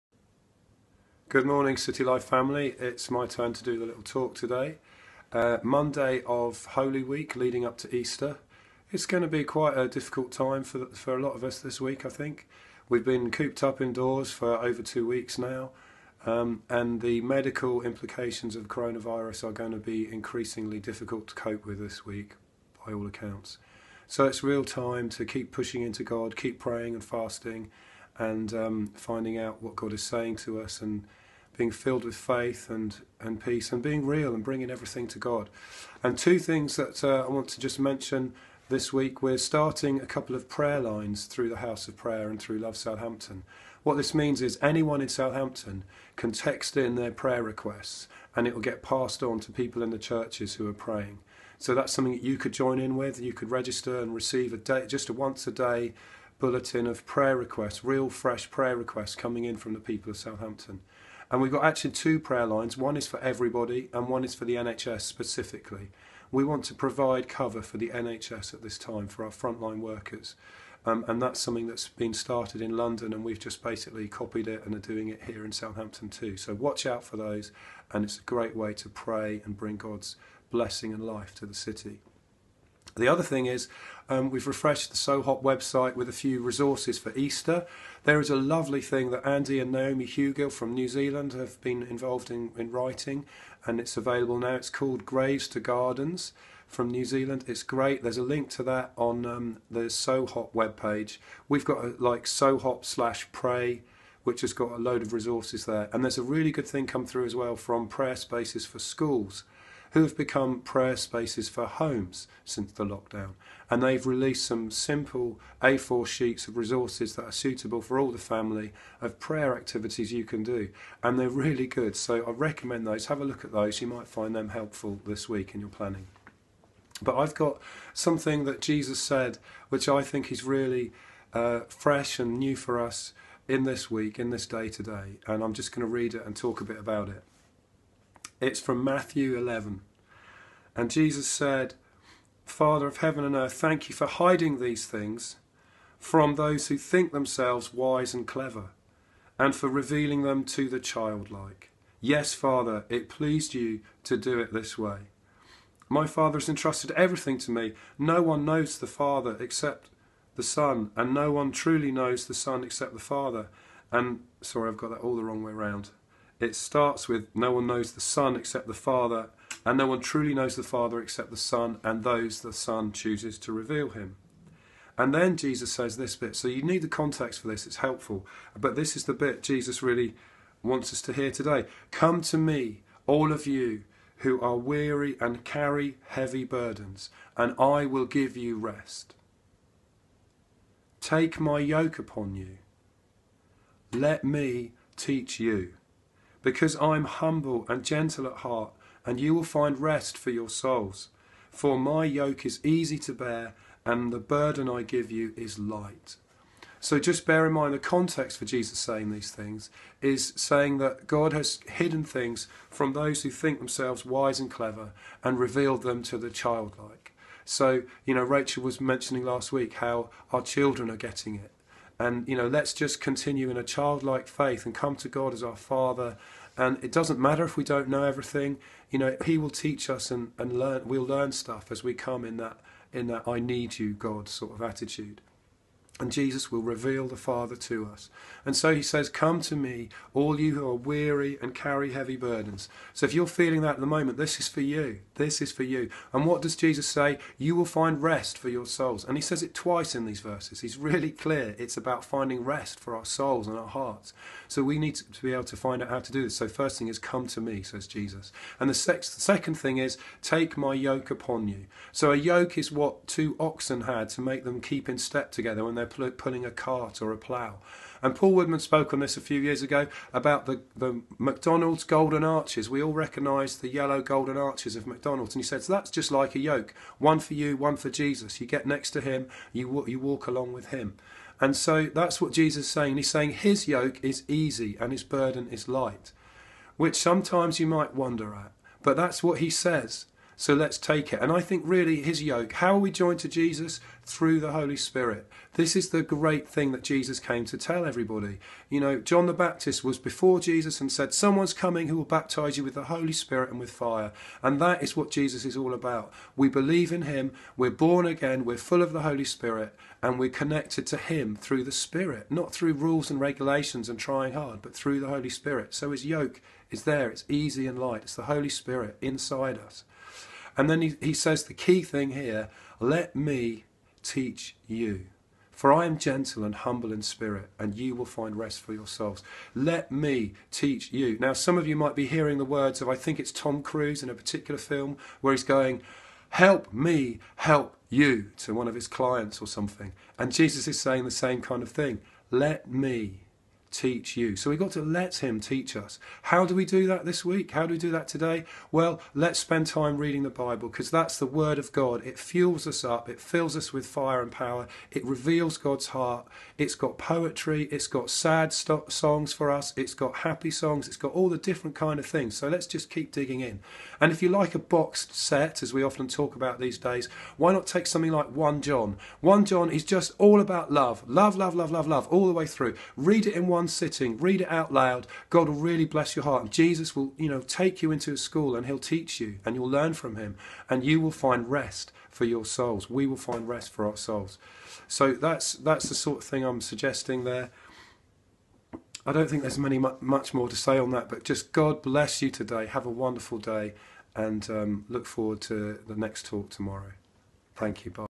Day 16: City Life 21 days of prayer and fasting
Rich encourages us to keep pushing into God, to keep praying and fasting as we enter Holy Week. He reminds us that in Matthew 11, Jesus invited all who are weary and carrying heavy burdens to come to Him and He will give us rest. Let’s come to Him today and keep in step with Him today!

Rich also mentions the Graves to Gardens project which Andy and Naomi Hugill, friends in New Zealand have been involved in putting together - it is a great resource to help us reflect as individuals and families, and engage with God across the Easter weekend.  Check it out here: gravestogardens.com, more resources can be found on the Southampton House of Prayer website (sohop.org)